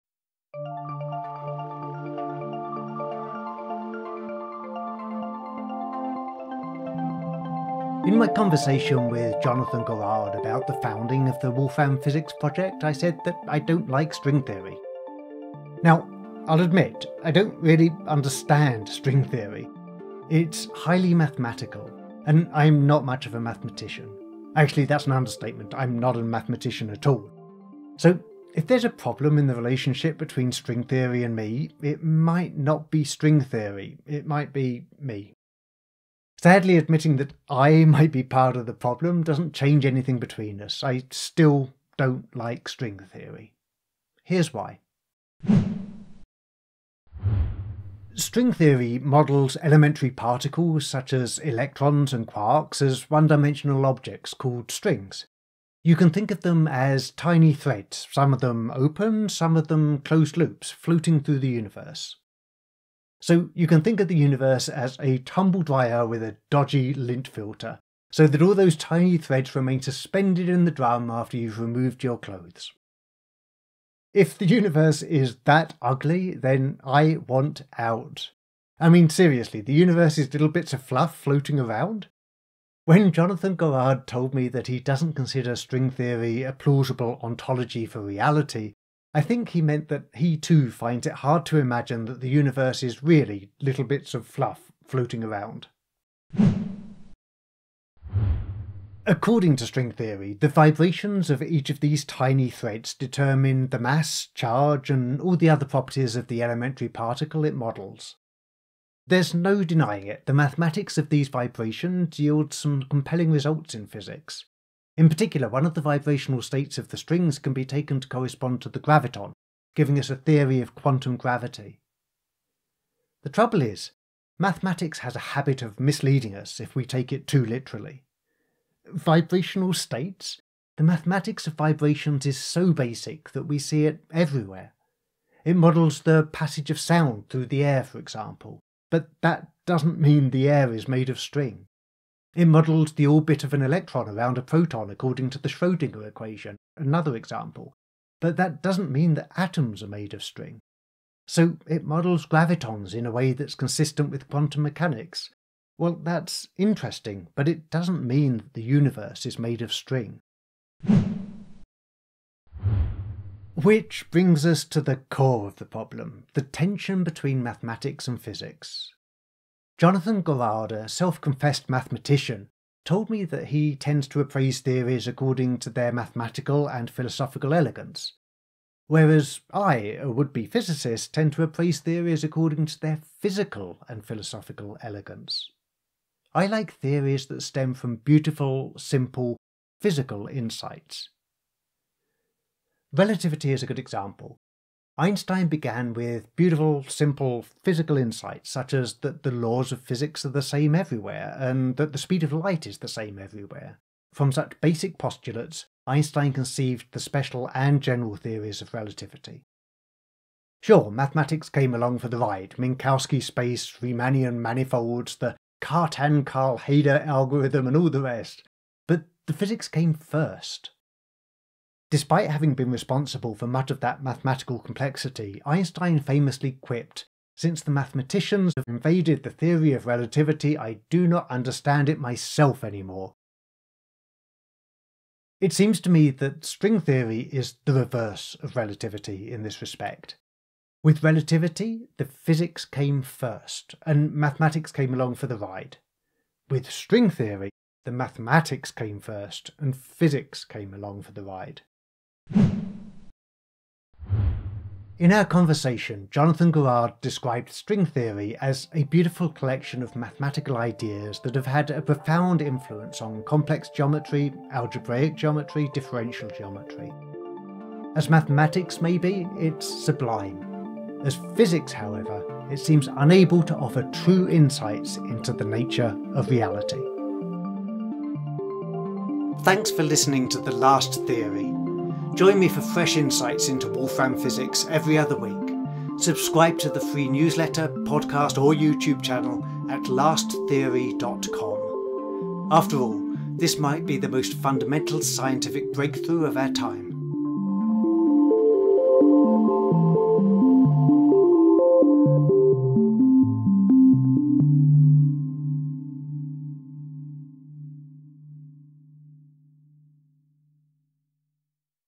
Why I don’t like String Theory
In my conversation with Jonathan Gorard about the founding of the Wolfram Physics Project, I said that I don’t like String Theory.

Now, I’ll admit, I don’t really understand String Theory.

It’s highly mathematical. And I’m not much of a mathematician. Actually, that’s an understatement. I’m not a mathematician at all.

So if there’s a problem in the relationship between String Theory and me, it might not be String Theory, it might be me.

Sadly, admitting that I might be part of the problem doesn’t change anything between us. I still don’t like String Theory.

Here’s why.

Prefer to listen to the audio? Search for The Last Theory in your podcast player, or listen

Kootenay Village Ventures Inc.